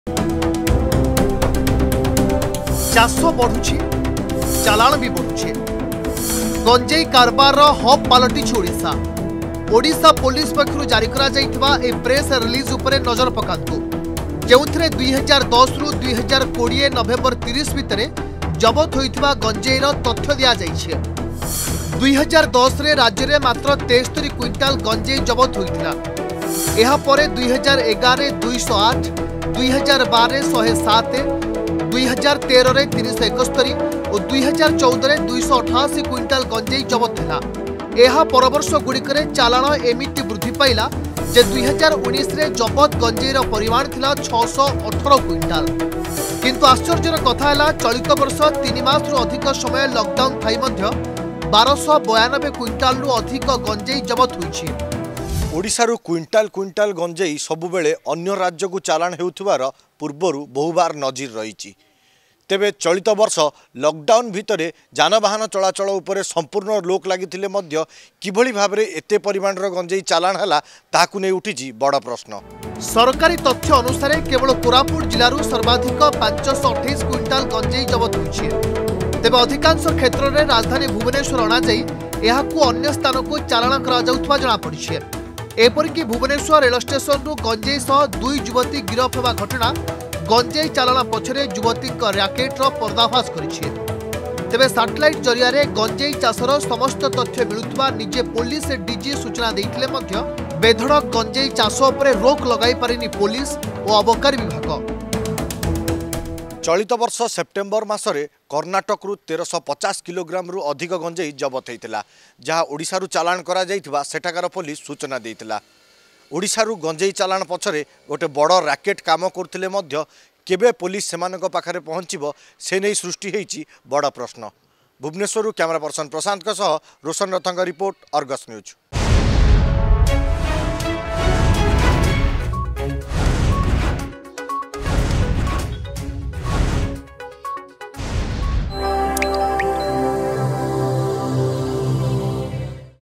ଗଞ୍ଜେଇ କାରବାରର ହବ ପାଲଟିଛି ଓଡିଶା
ଚଳିତ ବର୍ଷ ଜବତ ହୋଇଛି 1292 କ୍ବିଣ୍ଟାଲରୁ ଅଧିକ ଗଞ୍ଜେଇ । 2010 ମସିହାରେ ଜବତ ଗଞ୍ଜେଇର ପରିମାଣ 73 କ୍ବିଣ୍ଟାଲ ଥିଲା । 9 ବର୍ଷ ଭିତରେ ଚାଲାଣ ଏତେ ବଢ଼ିଯାଇଥିଲା ଯେ 2019ରେ ଜବତ ଗଞ୍ଜେଇର ପରିମାଣ ଥିଲା 618 କ୍ବିଣ୍ଟାଲ.